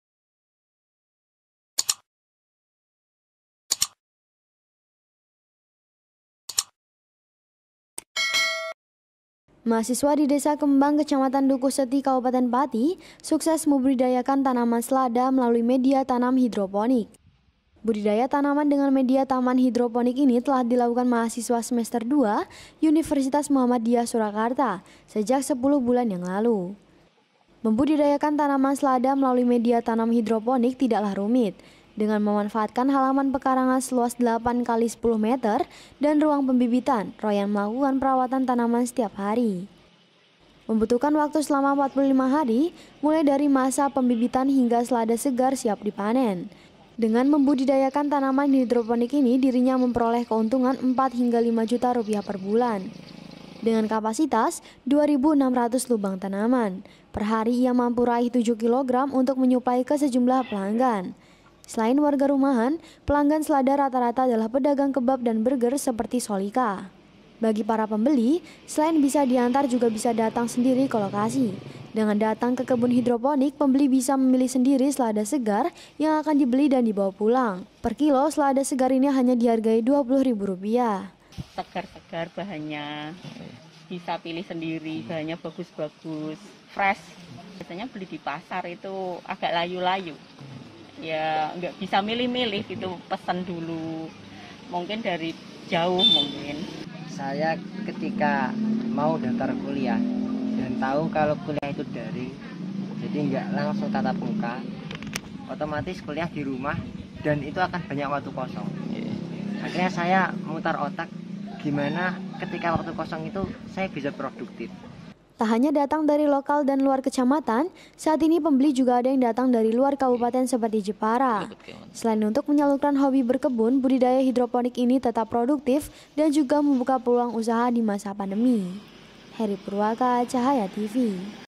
Mahasiswa di pati ini jadi petani milenial, di sela belajar daring
Belajar  daring, membuat siswa jenuh jika tidak ada aktivitas lain. namun tidak,bagi muhammad royyan, mahasiswa asal desa kembang,kecamatan dukuhseti kabupaten pati. mengisi waktu luang di sela belajar daring, justru membuat ia meraup omzet jutaan rupiah per bulan, dengan menjadi petani milenial menanam selada sistem hidroponik.